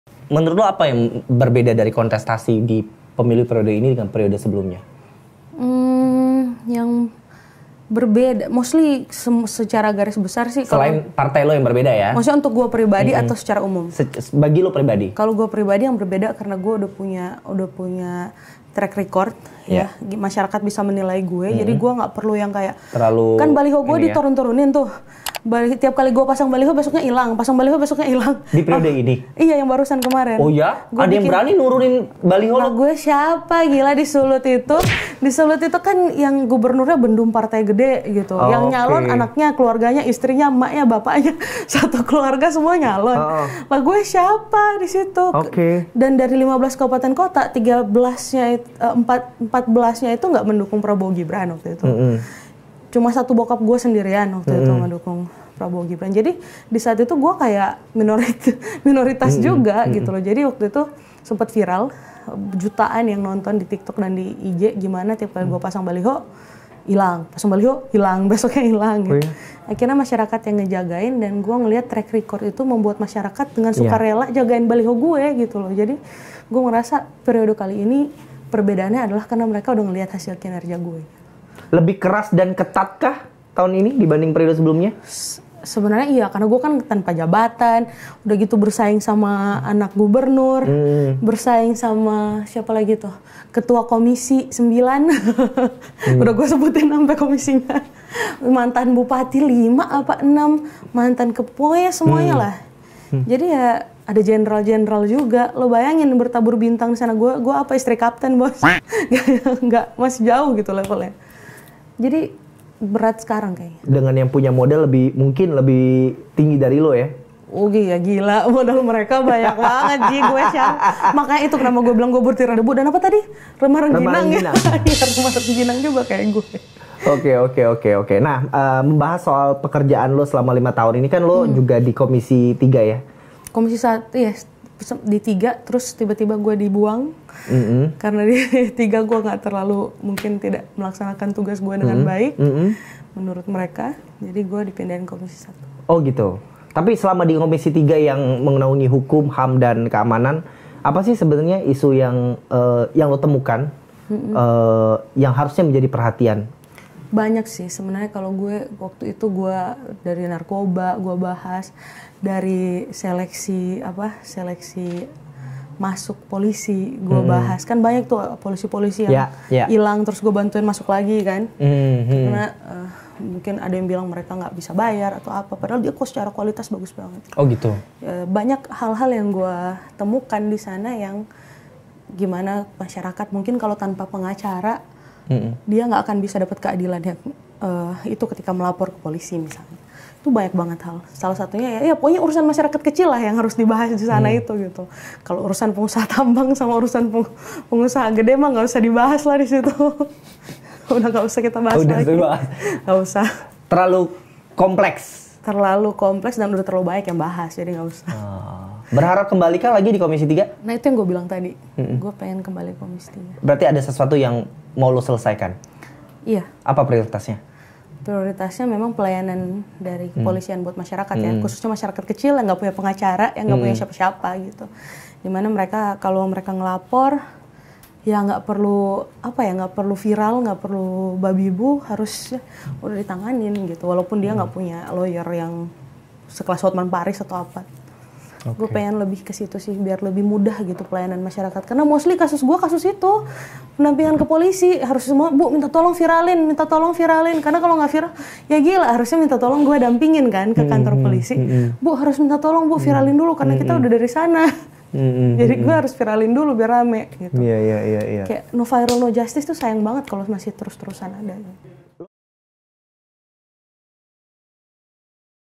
Baliho Hillary Lasut Hilang Saat Masa Kampanye Pemilu 2024 | PODCAST KODE
Hillary Lasut menjelaskan apa yang berbeda antara kontestasi pemilu di periode kali ini dengan periode sebelumnya.

Menurutnya, rakyat sudah memiliki kepercayaan padanya. Hal ini pun terbukti ketika baliho Hillary Lasut di Sulawesi Utara berkali-kali hilang, masyarakat yang mendukung Hillary secara sukarela menjaga Baliho tersebut agar tidak hilang lagi.

Hillary Lasut juga memaparkan hal-hal apa saja yang perlu menjadi perhatian khusus bagi DPR komisi III.